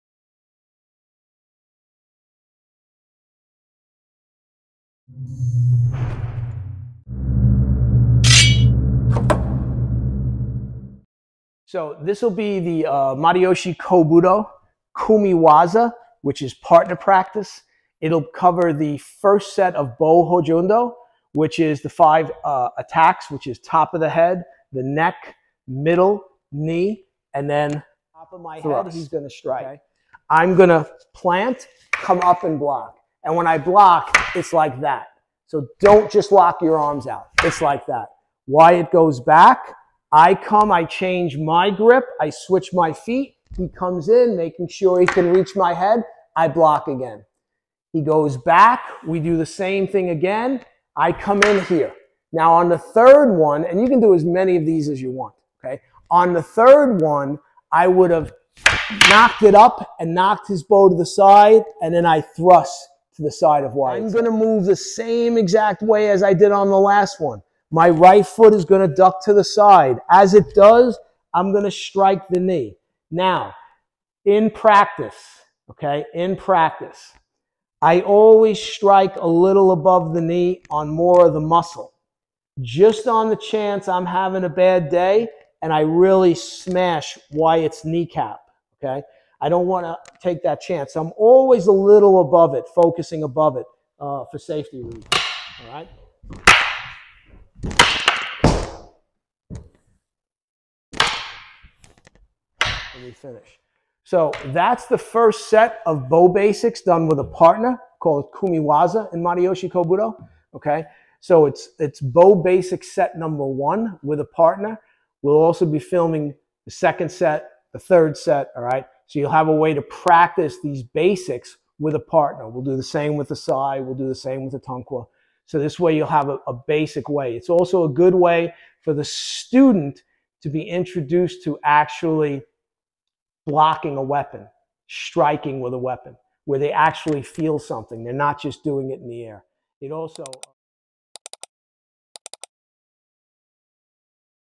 Bo Kumi Waza "Trailer" Matayoshi Kobudo Michael Calandra
I teach the Basic Bo/Bo Kumi Waza of Matayoshi Kobudo in this easy to follow presentation. This is the actual introduction to Kobudo partner practice.